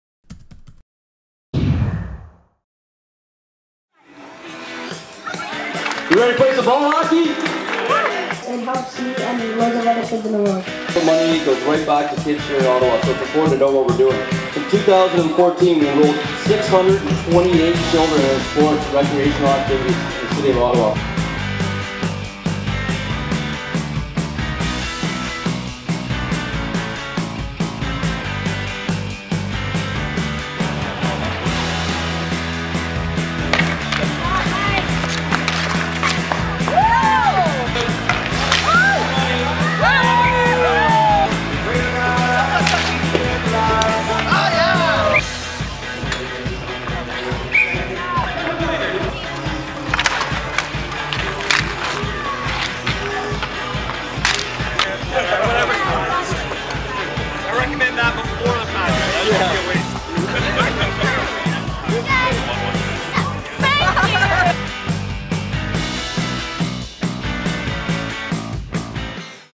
RLA Architects Blog #1
RLA Architects took part in the 4th annual Their Opportunity ball hockey tournament at Lansdowne Park Saturday August 8th. Check out some highlights!